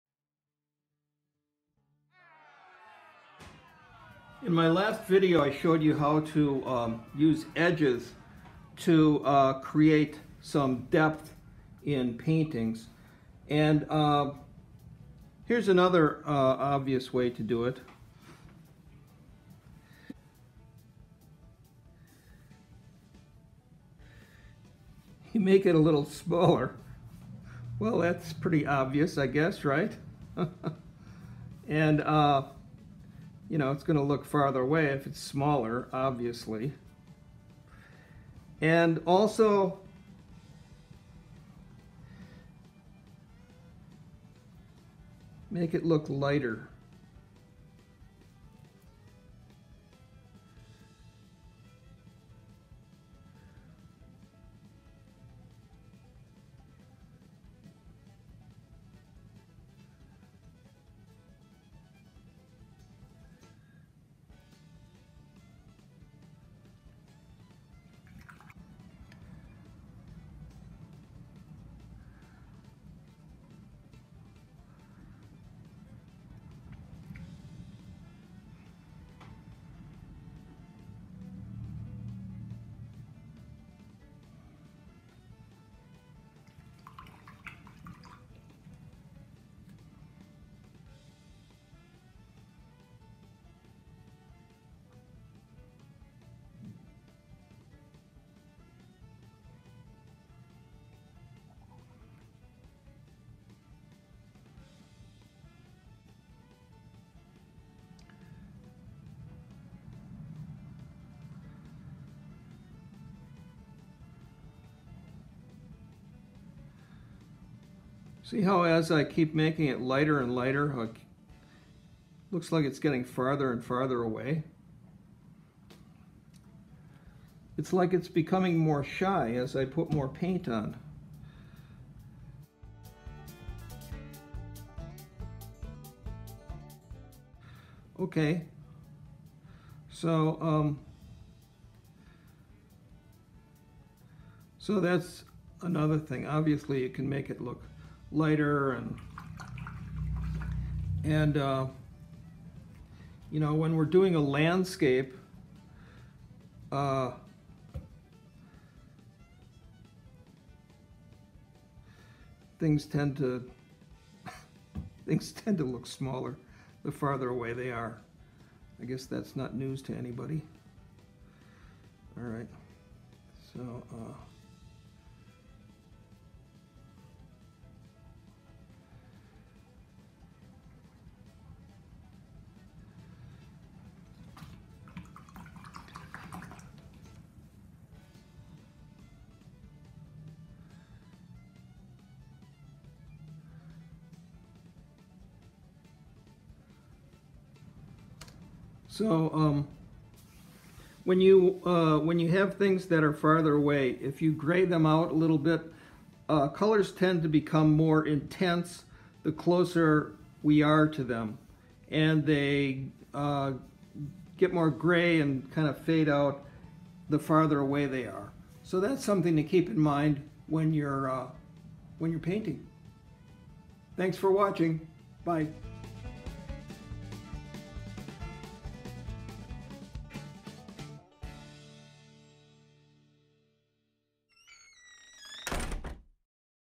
Rec. At Home, Paint With Fred, How to create depth in my paintings. June 17, 2020
Fred Bell shows how he creates the illusion of depth in his paintings.